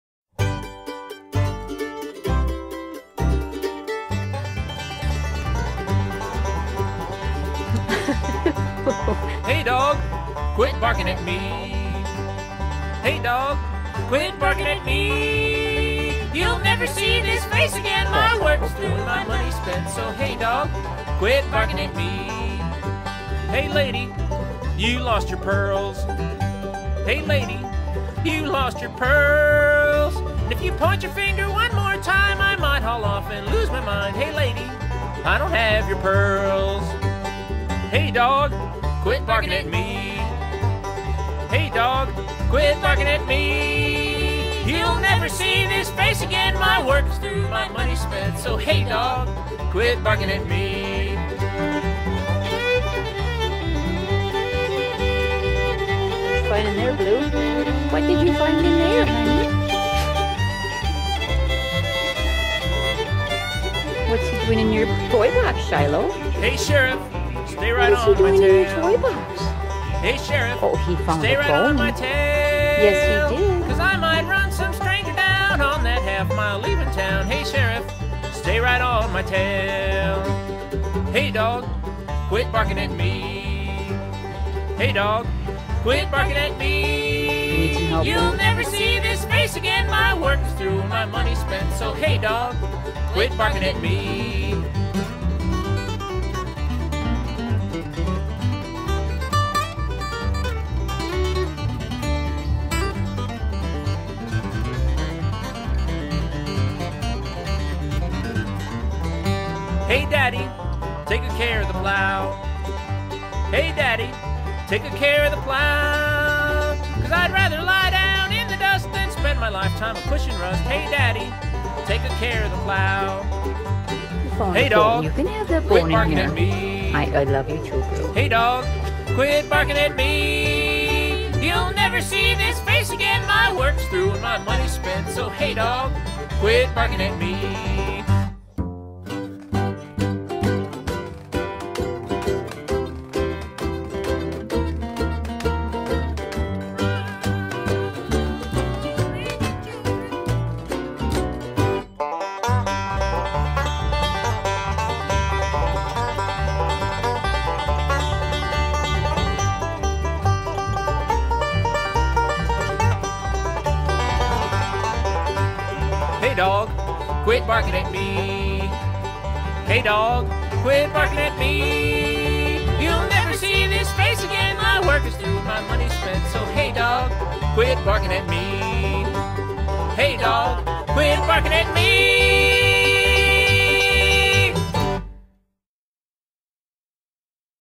What is it like having five dogs in an RV
Rving with our two dogs who meet up with two more dogs at a ranch in Texas, at Deer Lake Cabins. Then another dog shows up so now there are 5 dogs! The temperatures were the coldest they have been forever and I just couldn't let 3 dogs stay out in the below freezing temperatures...